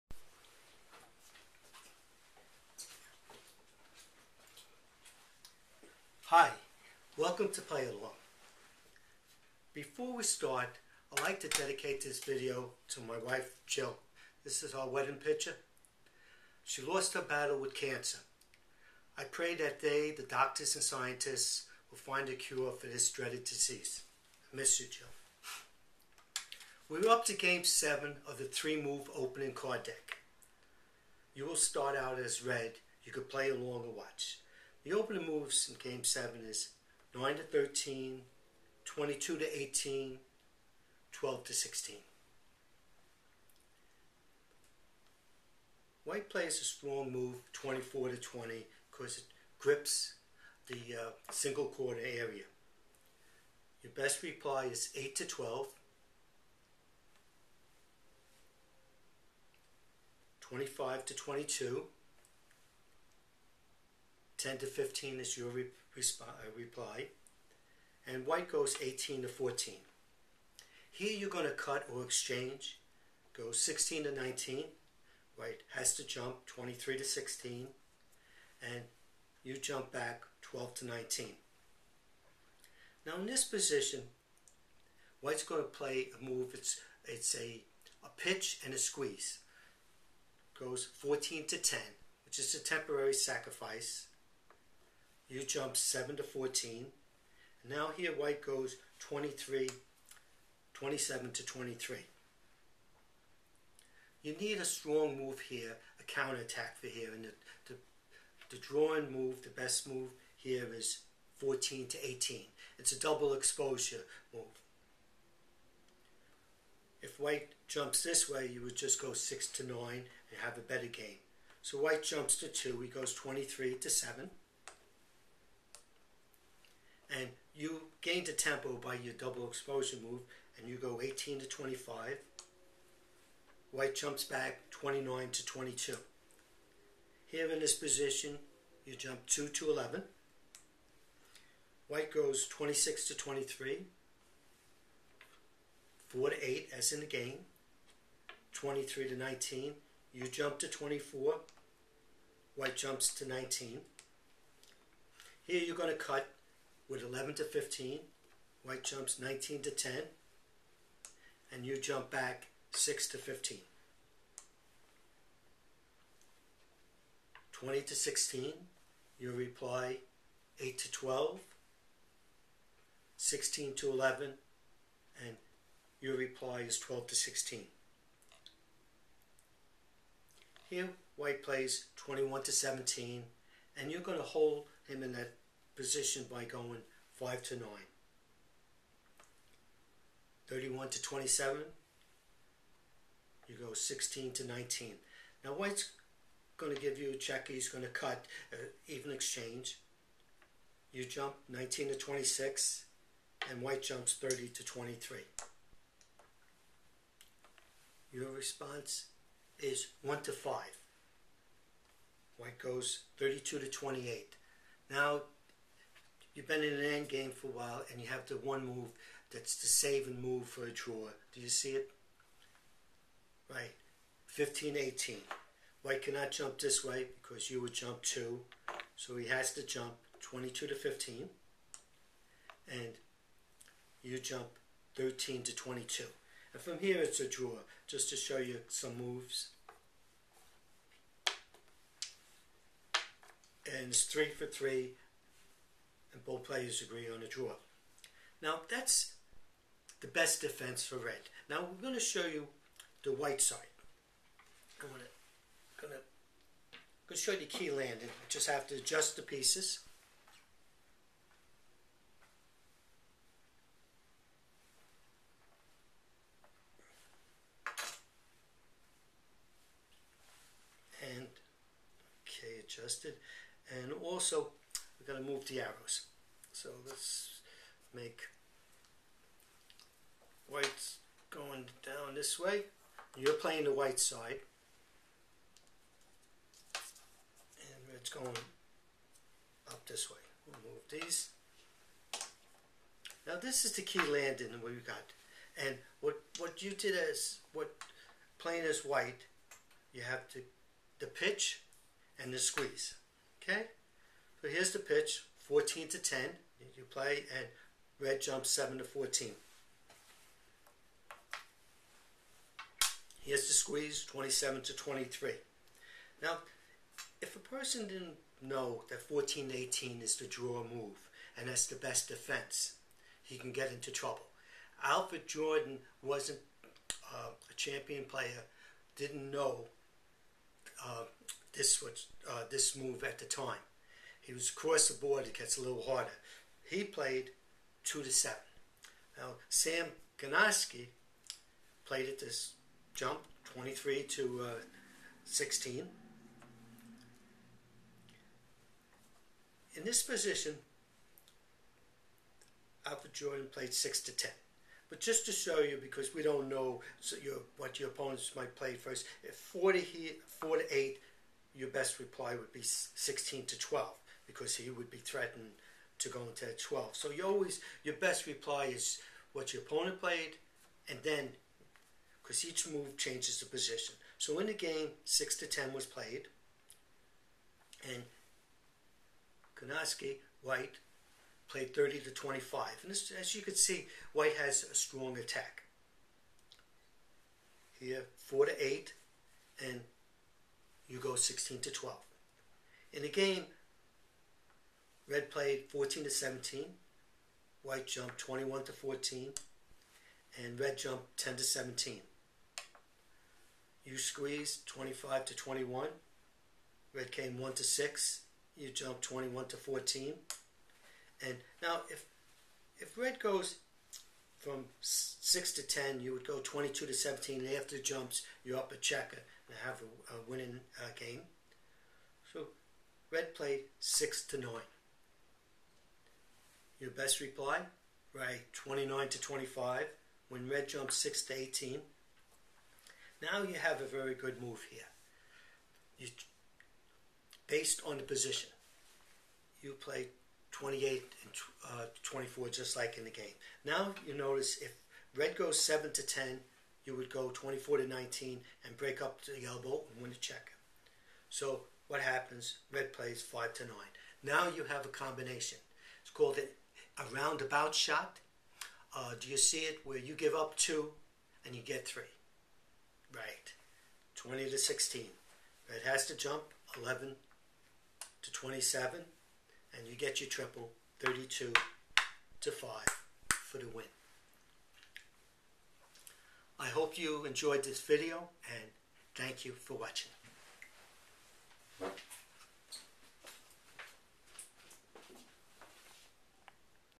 CHECKER CLASS..GAME 7
THE Edinburgh Opening.  We go over a famous game played by Alfred Jordan (red) vs Sam Gonotsky (white) where a new move was played making it a tough cross board game against a hard opening.  The pitch and the squeeze idea and the round about shot for a 3 for 2.